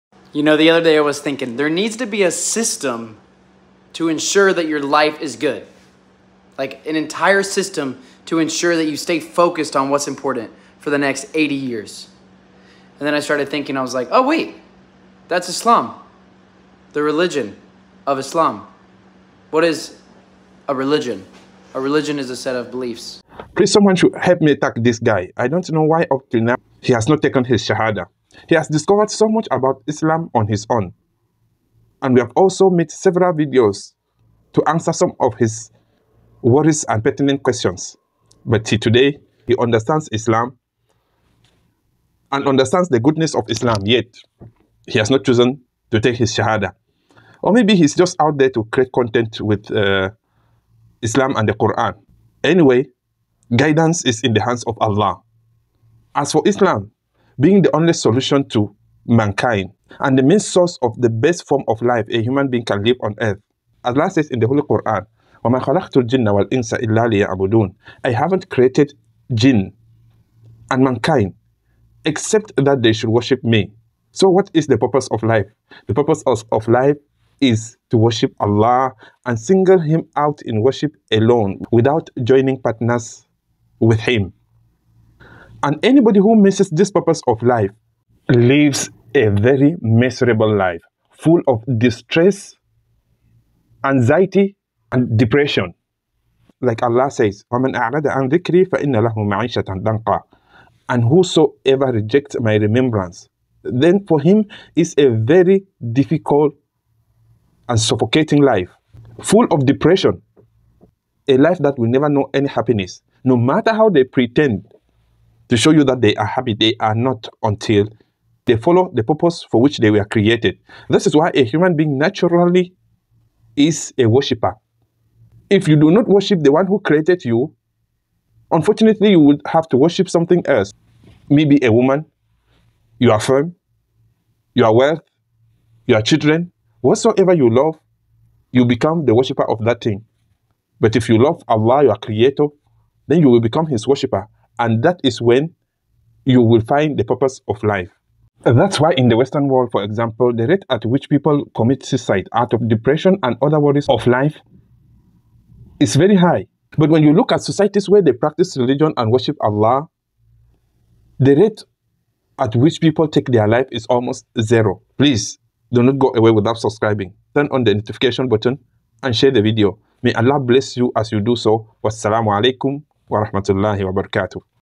Islam Is wrong & Doesn't Guarantees Happy Life?
The american boy @graysonbrockk who has been studying Islam for quite a while now, asks a question about the purpose of life, if Islam is able to guarantee a good purpose of life that will make one live happily for ever. @alhaarithislamictv339 guves him the best answer